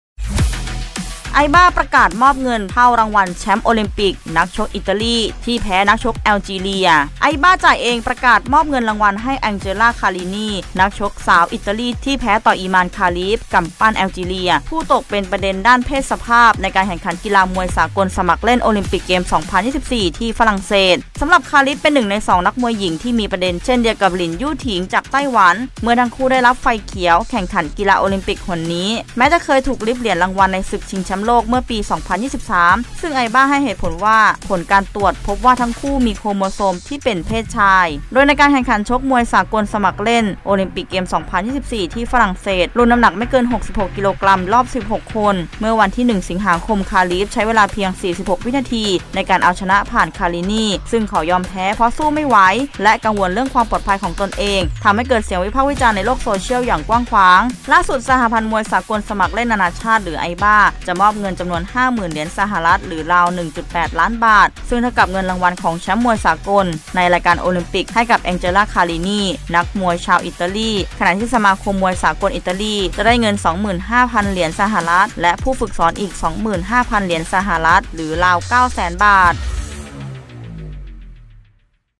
“ไอบา” ประกาศมอบเงินเท่ารางวัลแชมป์โอลิมปิกนักมวยอิตาลีที่แพ้นักชกแอลจีเรีย : 06-08-67 | INN Sports
“ไอบา” ประกาศมอบเงินรางวัลให้ “อังเจลา คารินี” นักชกสาวอิตาลี ที่แพ้ต่อ “อิมาน คาลิฟ” กำปั้นแอลจีเรีย ผู้ตกเป็นประเด็นด้านเพศสภาพ ในการแข่งขันกีฬามวยสากลสมัครเล่น โอลิมปิกเกมส์ 2024 ที่ฝรั่งเศส